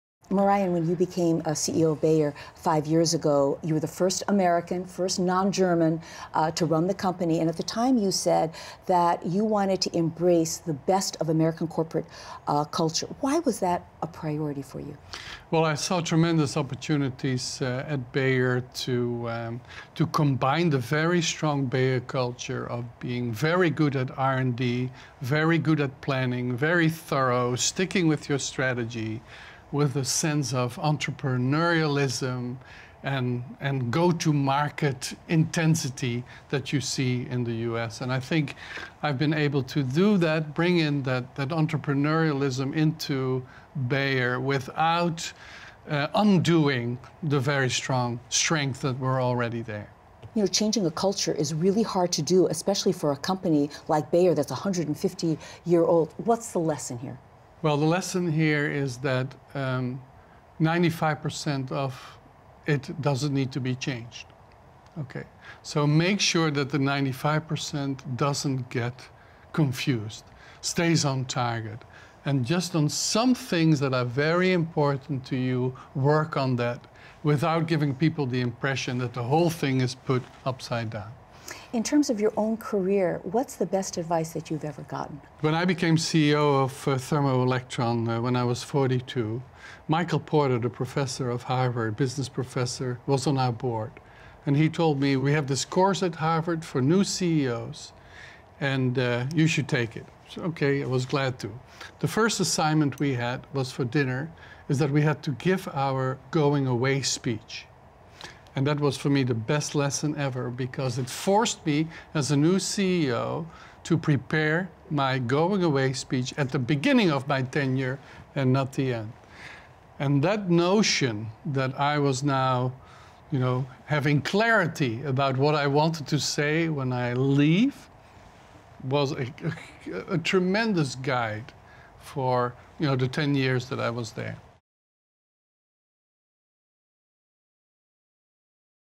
Bayer CEO: 'This Was the Best Lesson Ever' | Fortune
Marijn Dekkers tells Fortune's Susie Gharib his best advice.